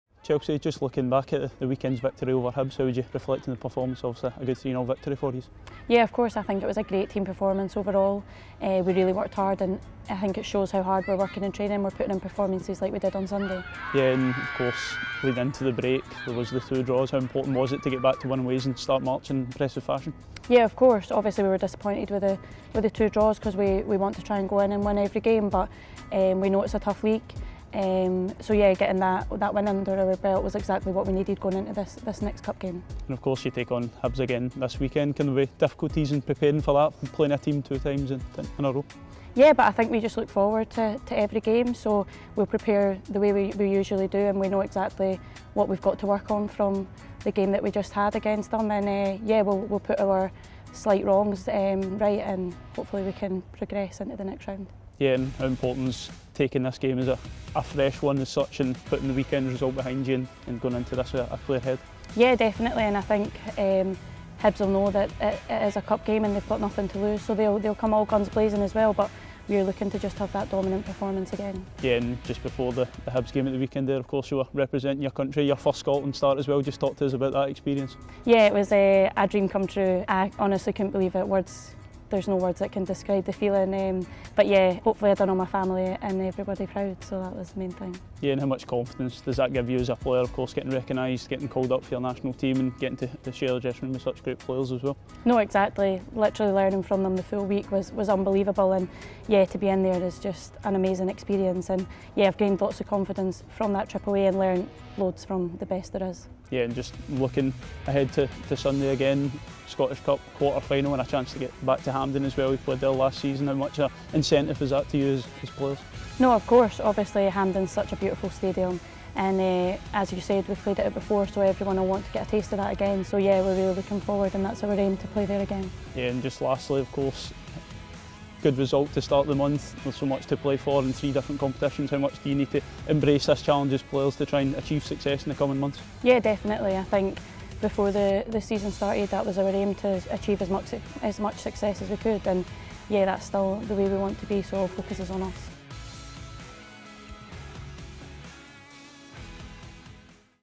INTERVIEW | Chelsea Cornet | Scottish Cup Duties in the Capital | 09 Mar 2024
Chelsea Cornet spoke to RangersTV ahead of their Scottish Cup quarter-finals fixture against Hibernian at Meadowbank Park.